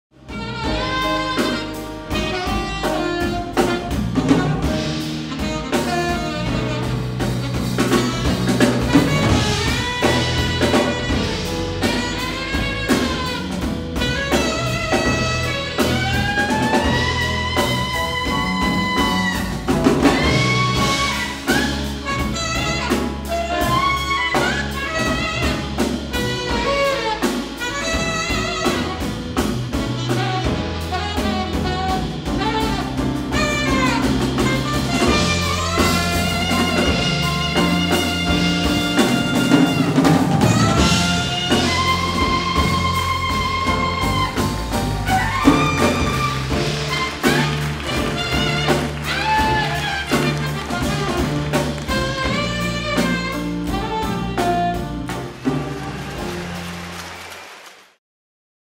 THANK YOU from Tri-C JazzFest Cleveland
The 39th annual Tri-C JazzFest, presented by KeyBank, was a huge success! Thank you to all who came out to Playhouse Square this year. Remember, you can always stick around with us year-round and stay up-to-date on all things jazz by signing up for our bi-weekly email newsletter.